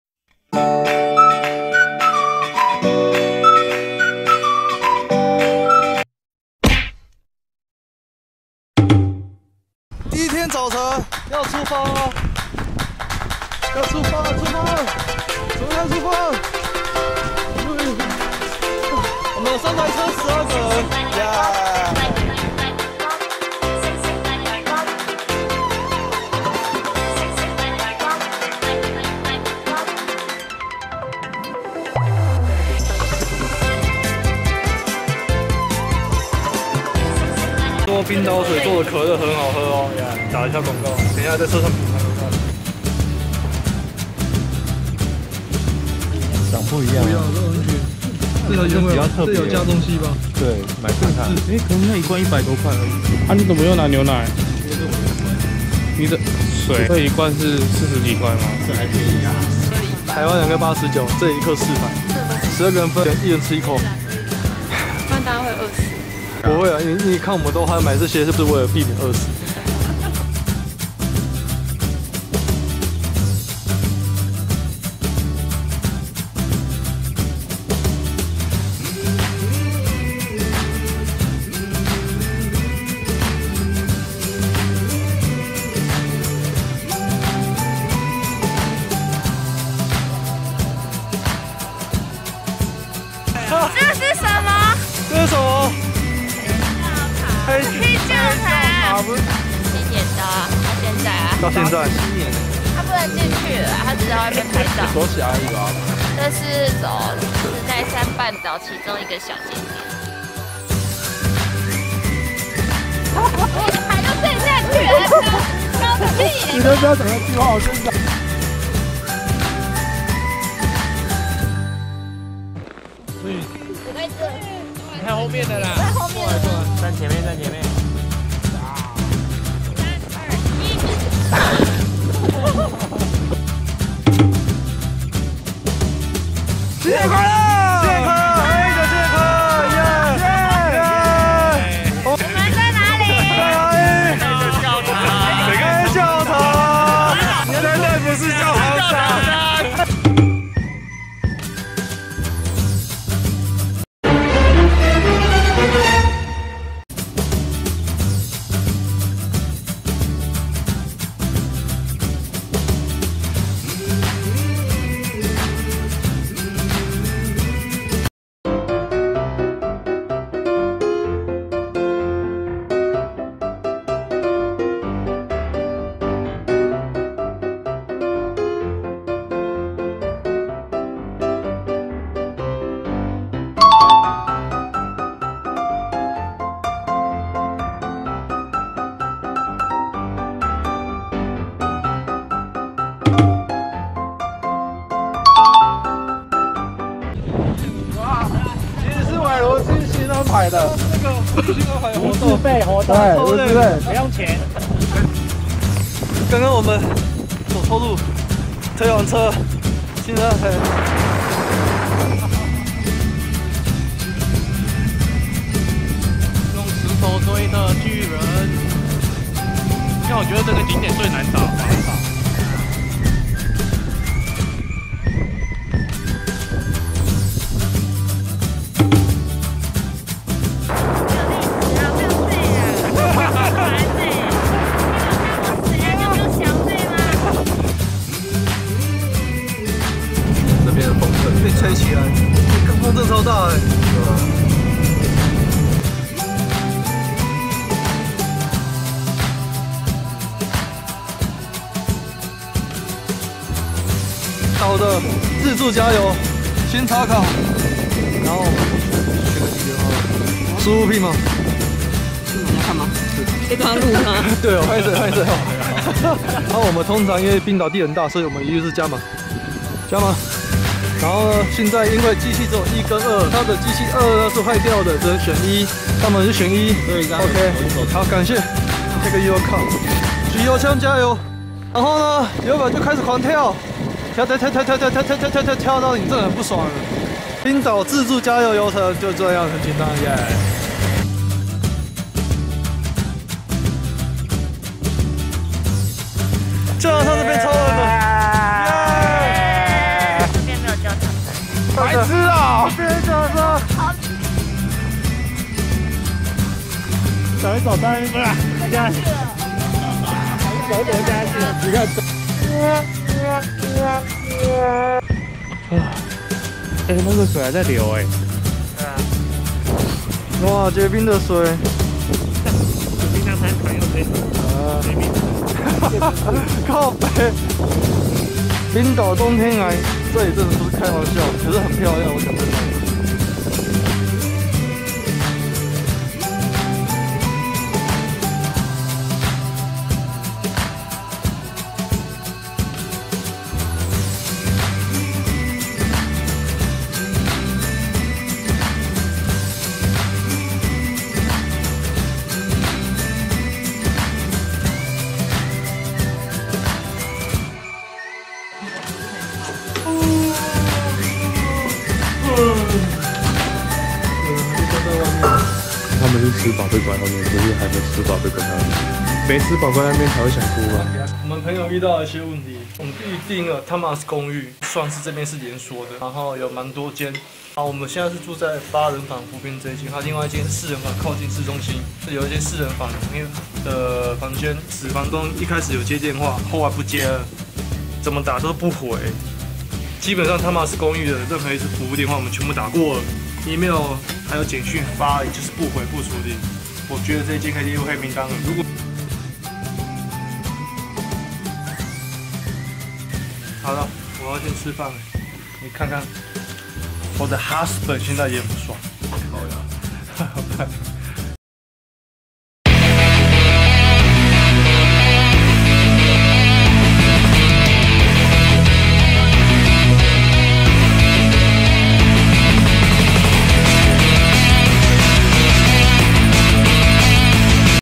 去旅行】冰島Day2 - 自駕卡雪地自救＋自助加油教學＋民宿被放鴿子怎辦！Iceland！冰島自由行GO【Ep2】
冰島Day2
我們自駕不小心卡在雪地了 , 好險人夠多大家想辦法把車子救起來了！！
瞬間體驗到友情可貴！！
雖然晚上被Thomsen公寓放鴿子 , 好險人多房間多 , 大家擠一擠就沒事情
去冰島訂房一定要特別小心！！